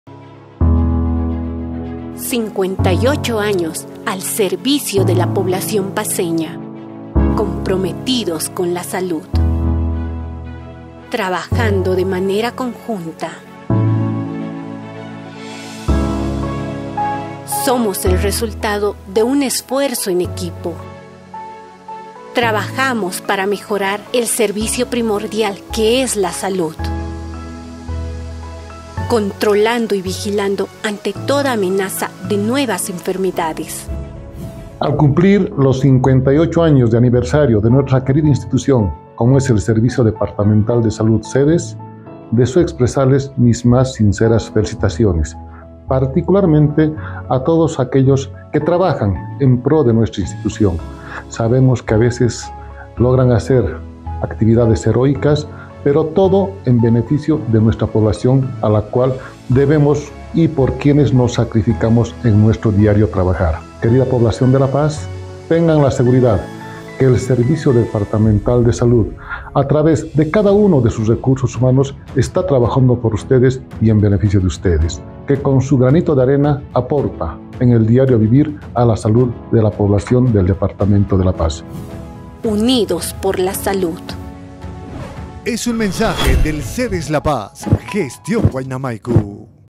VIDEO ANIVERSARIO SEDES 2023
Cumpliendo con el trabajo en bien de la salud con las 20 provincias y 87 municipios, el SEDES La Paz cumple 58 años de trayectoria siendo una institución rectora al servicio de la población paceña.
¡Feliz aniversario, SEDES La Paz!